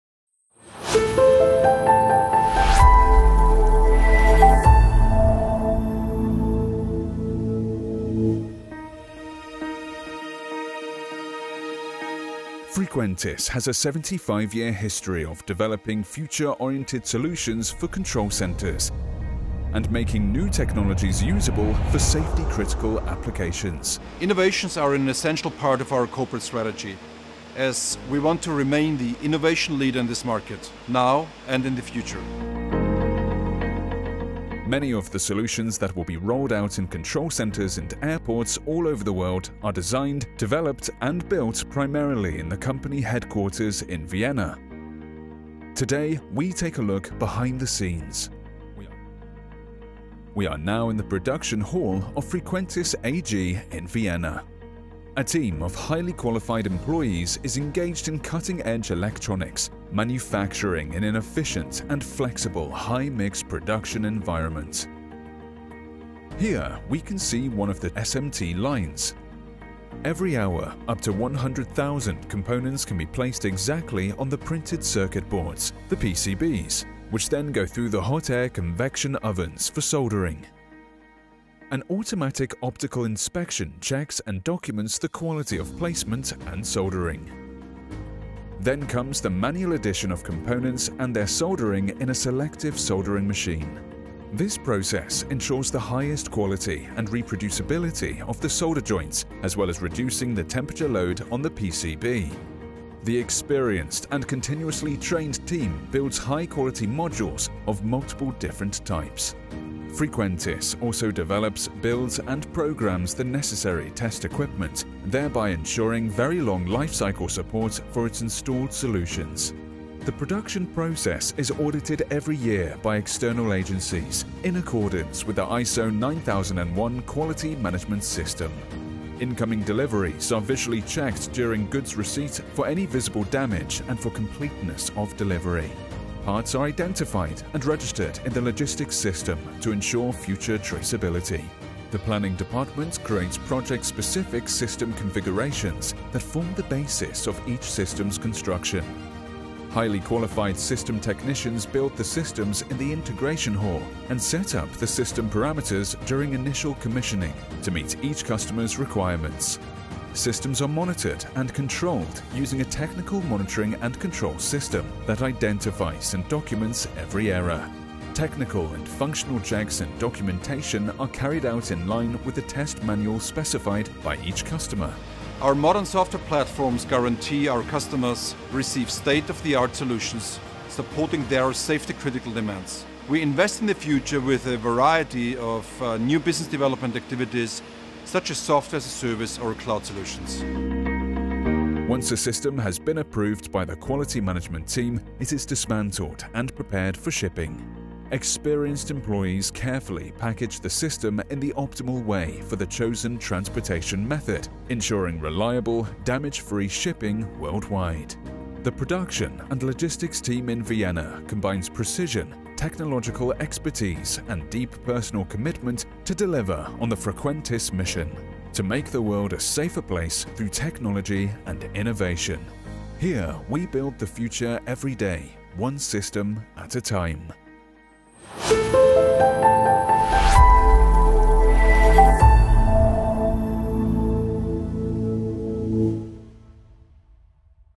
Frequentis production facilities tour
Customers and partners frequently visit Frequentis in Vienna. This is especially exciting, when it includes a tour through our production facilities. Many of the solutions that will be rolled out in control centres and airports all over the world are designed, developed and built primarily here, in our company headquarters. Since travel has been restricted recently, let us invite you to join us on a video tour through the facilities. Find out for yourself, how Frequentis technology is made. Enjoy!